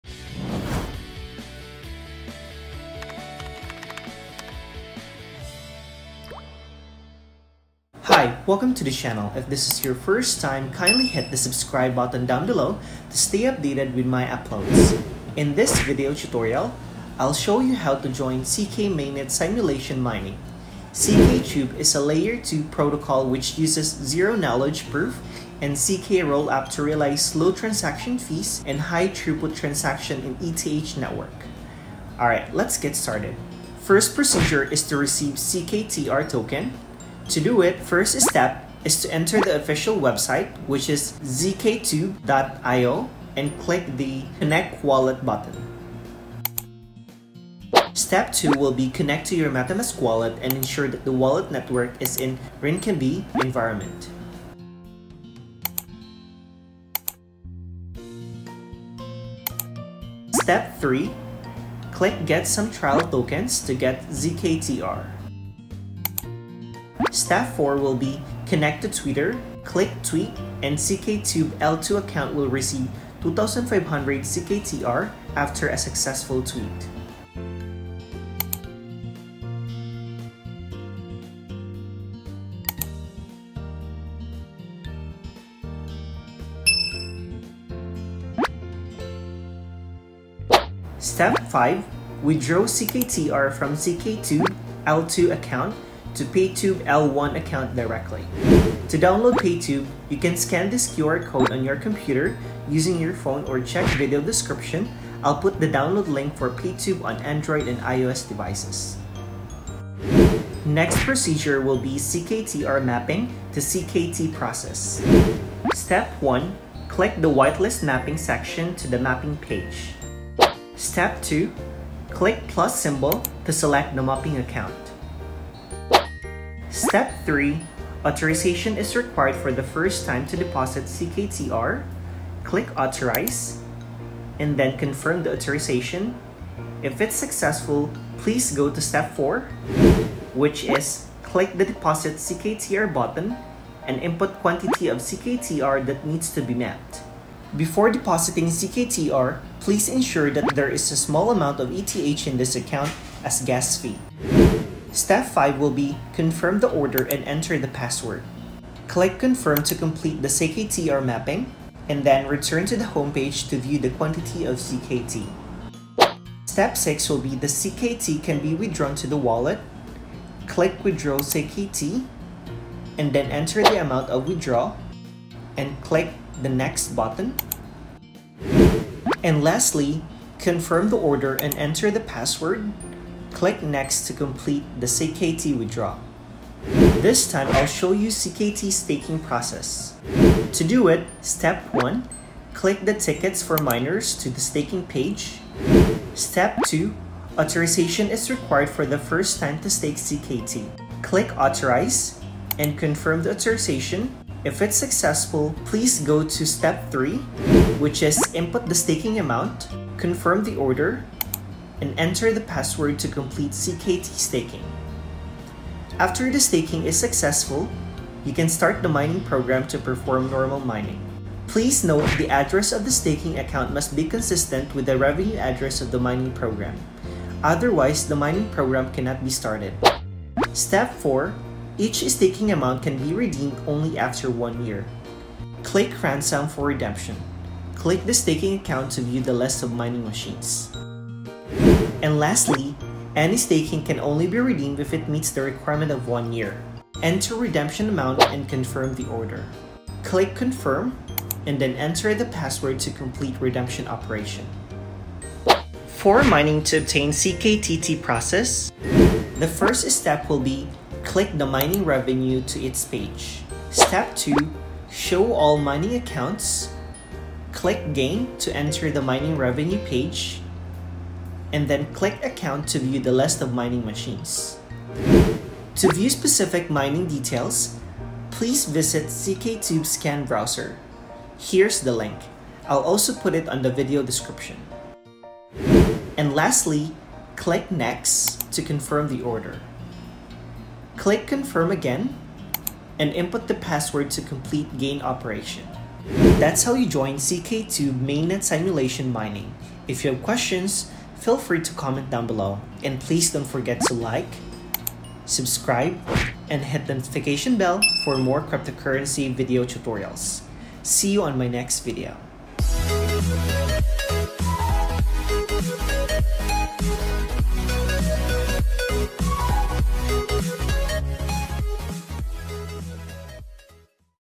zkTube Mainnet Simulation Mining Tutorial
Here’s a quick and easy tutorial on how you to join zkTube Mainnet Simulation Mining. See video to learn more!

⏰ Timestamp

00:35 - I. Steps to Receive ZKTR
01:50 - ZKTR Mapping to ZKT Process
03:08 - ZKT Staking Process
04:16 - Mining to obtain ZKTT Process

⬇️ Download Paytube Wallet

😎 View Specific Mining Details